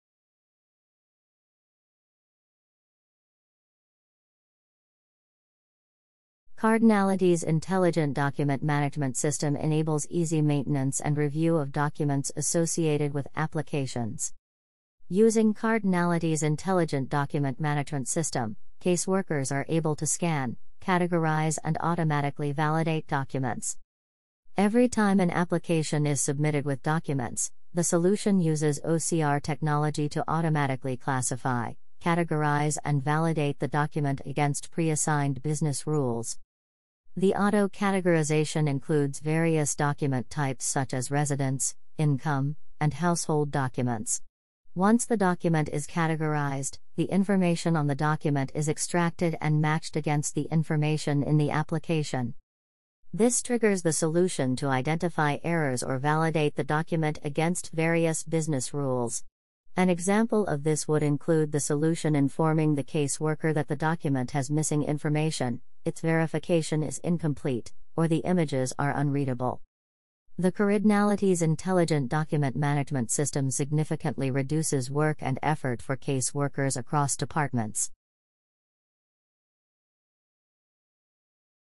Intelligent Document Management System
In this video, we talk about how our system transforms document handling for agencies. Watch as caseworkers effortlessly scan, categorize, and validate documents using cutting-edge OCR technology. Our solution automatically matches document information with application data, alerting caseworkers about errors and incomplete verifications in real-time. Experience the power of automatic document validation against pre-set business rules, enhancing accuracy and compliance.